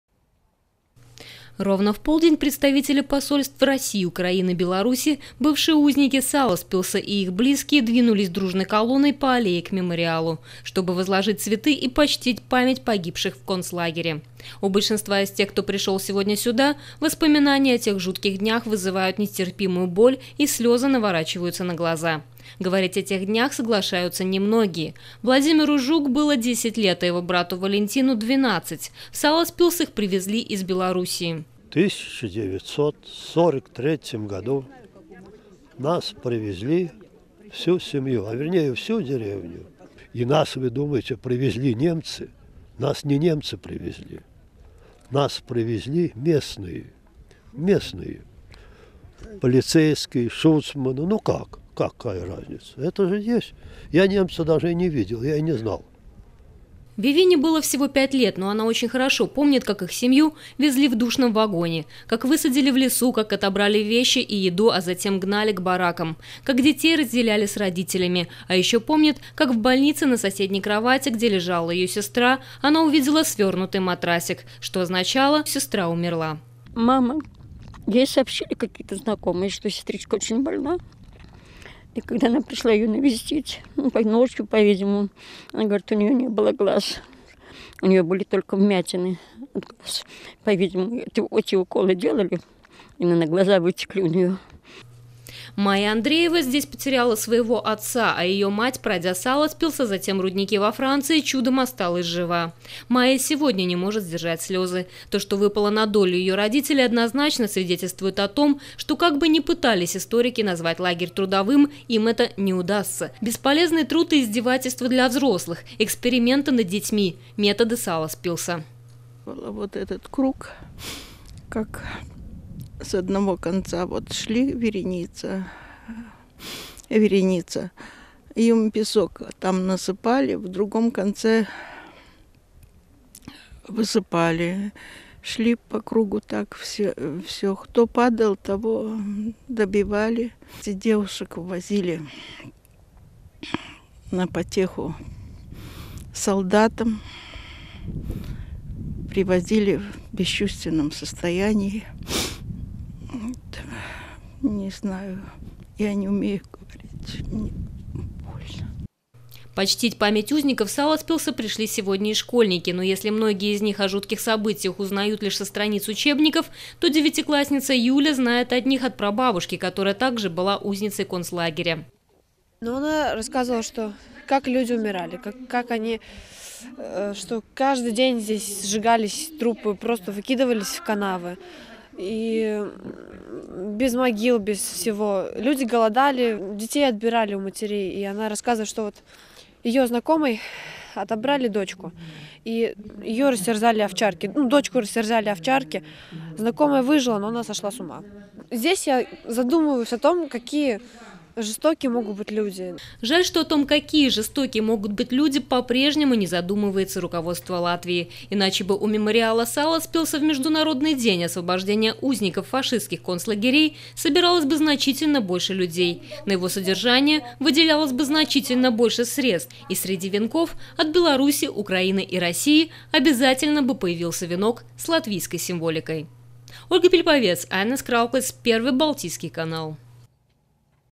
Репортаж Первого Балтийского канала
Репортаж Первого Балтийского канала о памятных мероприятиях приуроченных к Международному дню освобождения узников фашистских лагерей 11 апреля 2011 г.